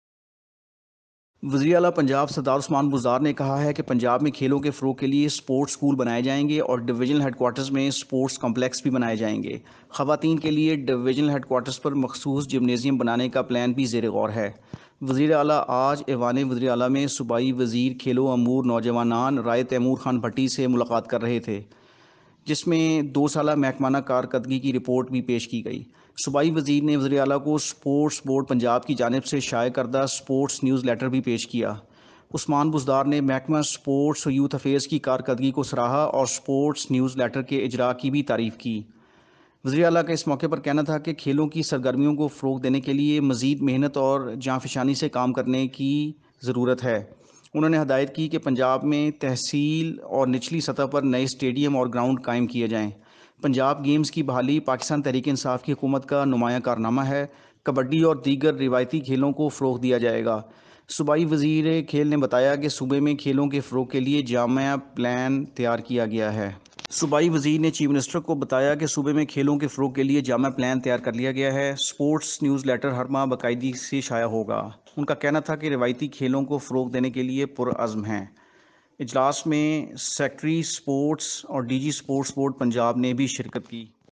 CM Buzdar announces constructing sports schools across Punjab
LAHORE-- CM Buzdar announces constructing sports schools across Punjab